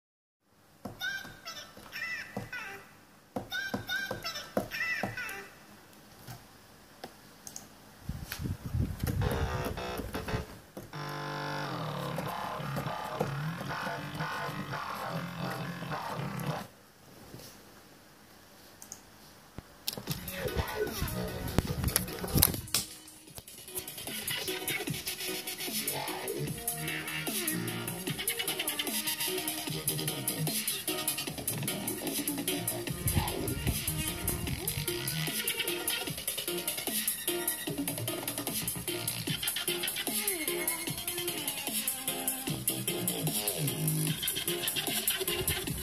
NIght dubstep sound design time
Practiced my sound design skills on serum and massive more.

Dubstep growls, drones and screeches, it usually helps to create a beat and some sort of small melody & lead too in order to see how the sound designs sound like in an ongoing music, even if it's not intended to be a full track. Then one can simply save the individual designs for later that could be useful in a full track or start doing a full song on the basis of the sound design project.

Also some vocal chopping got involved but mostly just practicing sound designs